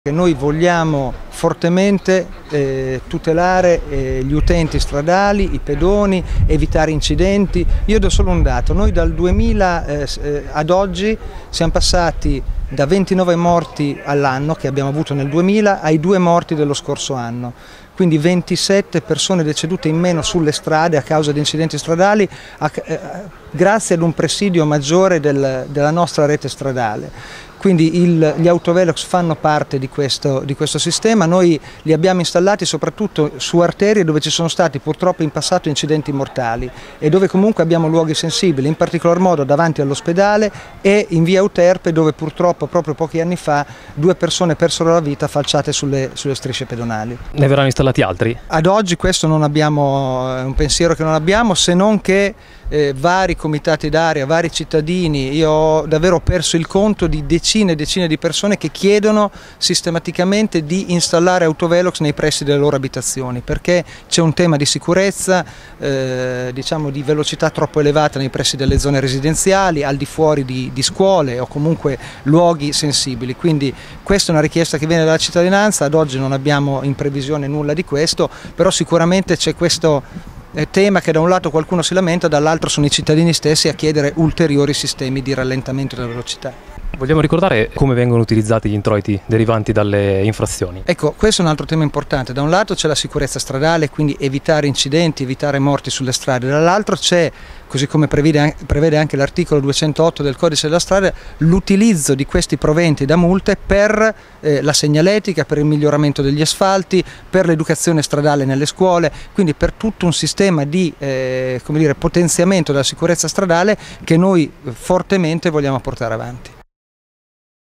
Autovelox, Sadegholvaad: "Non per fare cassa ma per tutelare i cittadini"
“Nessuna volontà di far cassa, la nostra priorità è esclusivamente la tutela dei cittadini”. E’ quanto affermato dall’assessore alla Sicurezza del Comune di Rimini, Jamil Sadegholvaad, in risposta alle accuse arrivate ieri, in commissione consiliare, da Gioenzo Renzi (Fdi) e Gennaro Mauro (Uniti si vince). Dito puntato contro gli autovelox e l’amministrazione comunale che, a detta dei due esponenti di opposizione, utilizza tali strumenti al solo scopo di incrementare i bilanci comunali. “Grazie a un presidio maggiore della nostra rete stradale siamo passati dai 29 morti sulla strada del 2000 alle 2 vittime dello scorso anno – ha affermato Sadegholvaad -. Tali dispositivi sono stati installati su arterie teatro di incidenti mortali o in luoghi sensibili, quali l’ospedale o via Euterpe dove pochi anni fa due persone persero la vita perché falciate sulle strisce pedonali”. L’assessore ha poi ricordato come vengono utilizzati gli introiti derivanti dalle infrazioni. “Come previsto dal codice della strada, per il miglioramento degli asfalti, la segnaletica e l’educazione stradale nelle scuole”.